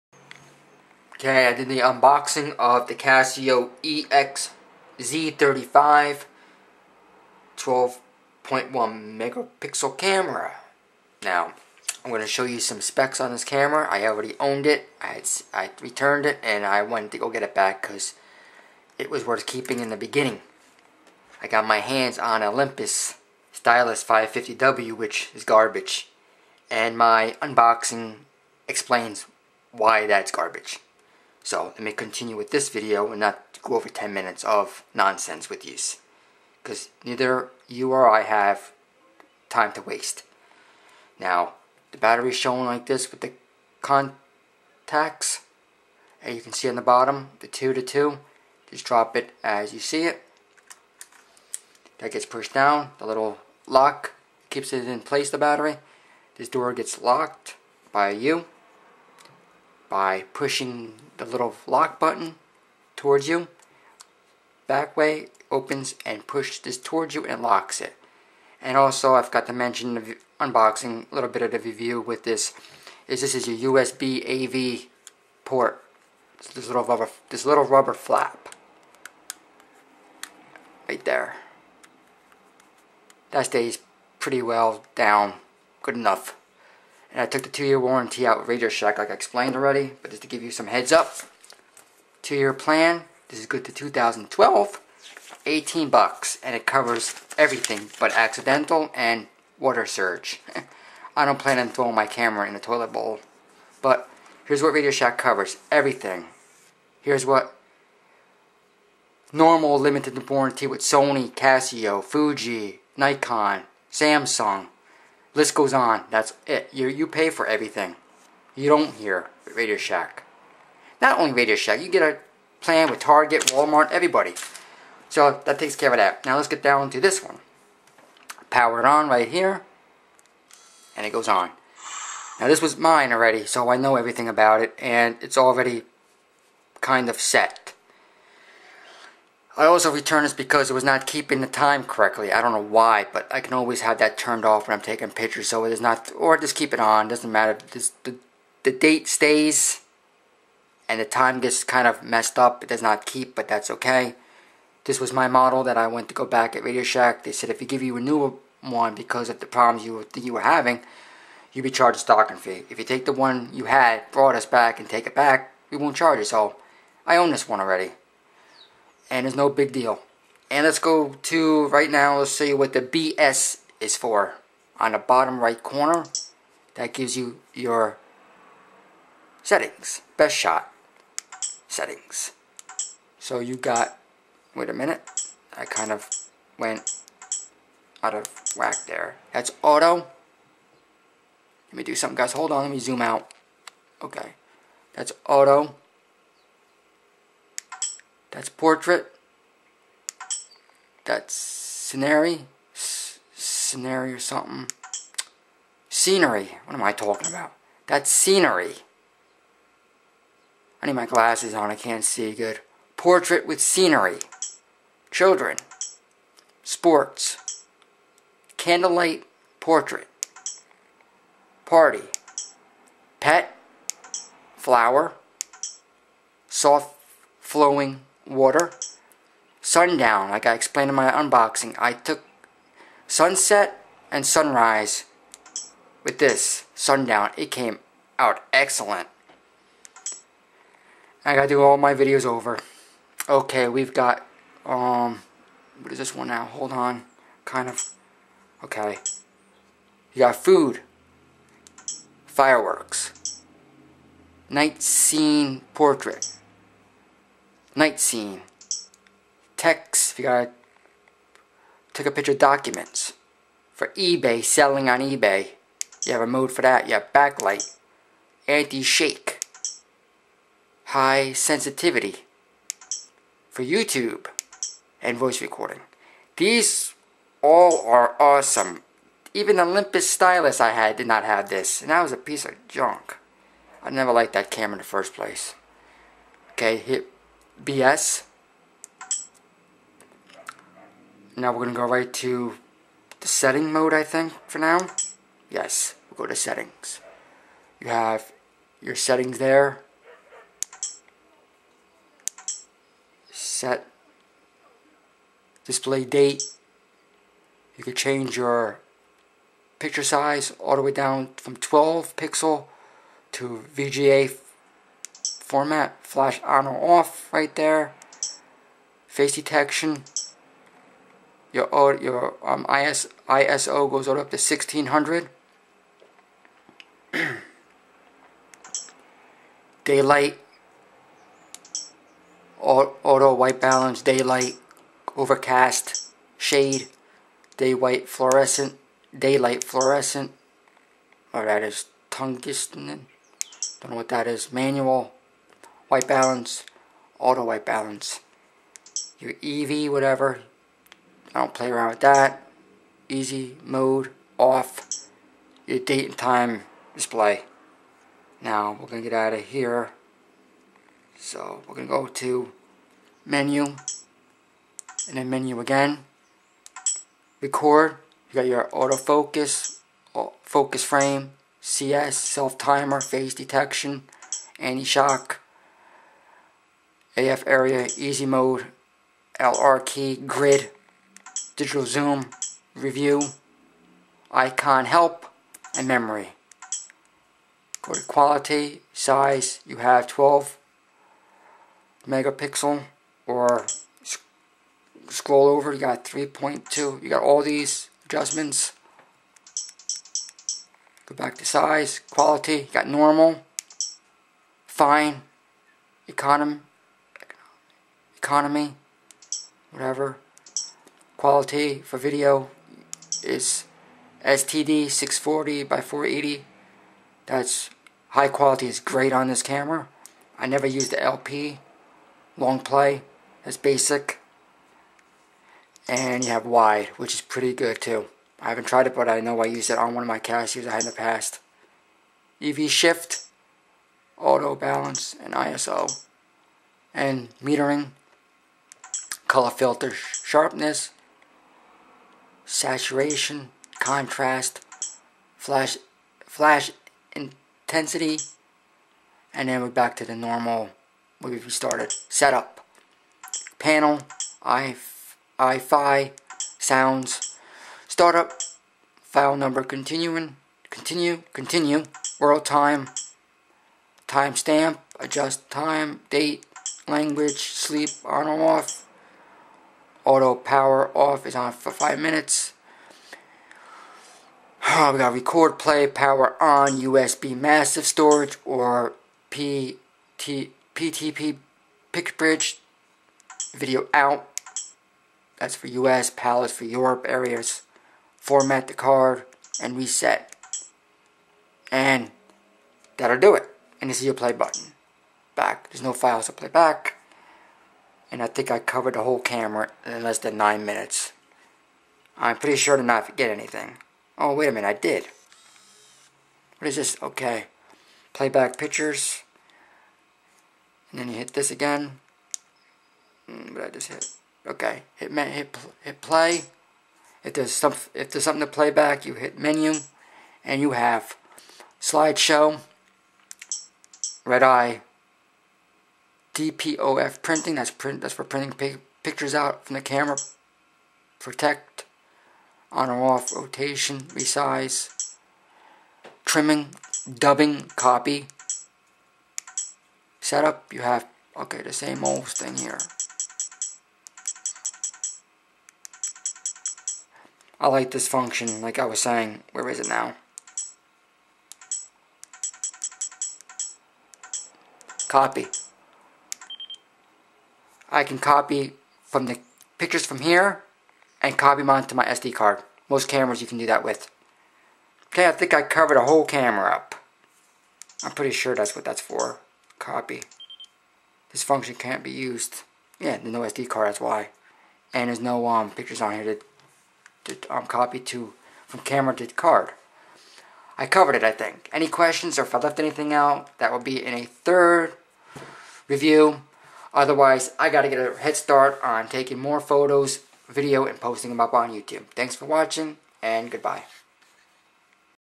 April 12, 2010 Review on Casio EX-Z35 Camera
Review on the Casio EX-Z35 12.1 pixel camera.